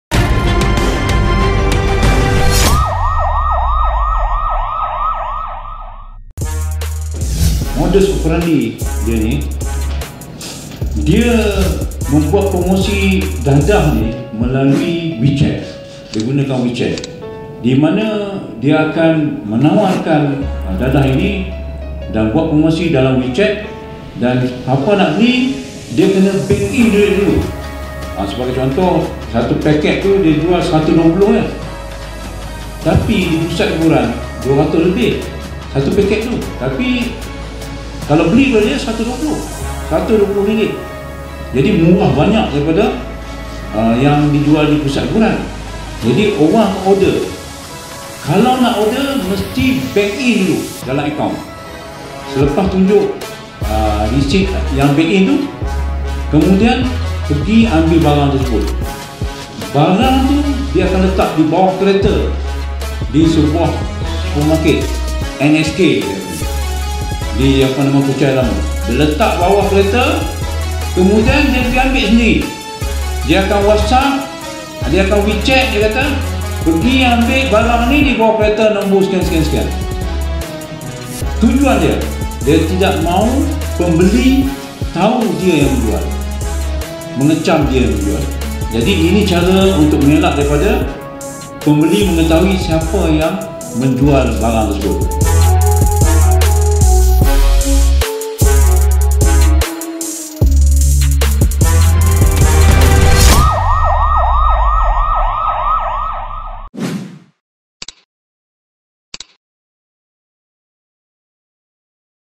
Sindiket Guna Teknik Baru Untuk Edar Dadah
Sindiket dadah di negara ini dikesan menggunakan modus operandi baharu dengan meletakkan dadah yang dibeli pelanggannya menerusi aplikasi Wechat di bawah mana-mana kereta secara rawak bagi mengelakkan kegiatan mereka dikesan pihak berkuasa.

Difahamkan, sindiket ini akan memasukkan sendiri dadah ke dalam paket minuman sebelum mempromosikannya di laman sosial Wechat dengan harga jualan serendah RM120 berbanding RM200 di premis hiburan.

Sebaik menerima wang pembayaran, sindiket tersebut akan meletakkan dadah di bawah mana-mana kereta di sekitar pasar raya sebelum diambil sendiri oleh pelanggan mereka.

Sindiket yang baru beroperasi sejak tiga bulan lalu itu didalangi sepasang kekasih warga tempatan, masing-masing berusia 40 tahun dan 28 tahun.

Ketua Polis Kuala Lumpur, Datuk Seri Mazlan Lazim berkata, penahanan kedua-dua suspek membawa kepada penemuan pelbagai jenis dadah bernilai RM590,779.
Beliau berkata demikian ketika sidang akhbar di Ibu Pejabat Polis Kontinjen Kuala Lumpur siang tadi.

Menurut Mazlan Lazim, suspek kemudian telah membawa sepasukan polis ke sebuah unit rumah di kondominium terbabit sebelum menahan seorang lelaki berusia 40 tahun.

Jelasnya, Menerusi serbuan tersebut, polis telah berjaya merampas 74 paket plastik dipercayai mengandungi dadah jenis MDMA, 140 pil ekstasi, 230 pil Erimin 5 dan satu paket plastik dadah jenis Ketamin.

Hasil siasatan mendapati kedua-dua suspek mempunyai rekod jenayah lampau berkaitan dadah dan mereka juga positif dadah jenis Methamphetamine dan Ketamin,.

Dalam serbuan berasingan, seorang lelaki warga tempatan berusia 27 tahun telah ditahan di sebuah unit rumah di sekitar Kuchai Avenue pada pukul 6.45 petang, Sabtu lalu.

Pihak polis telah berjaya merampas 14 paket plastik dipercayai mengandungi dadah jenis Methamphetamine bernilai RM40,200.

Hasil soal siasat mendapati, suspek aktif dalam kegiatan pengedaran dadah sejak dua bulan lalu di sekitar kawasan Lembah Klang dan ujian saringan air kencing mendapati suspek positif Meth.

Tambah Mazlan, pihaknya percaya dengan tertangkapnya ketiga-tiga suspek telah berjaya melumpuhkan aktiviti pengedaran dadah yang menggunakan modus operandi baharu di sekitar lembah Klang.

Kedua-dua kes akan disiasat dibawah Seksyen 39B Akta Dadah Berbahaya 1952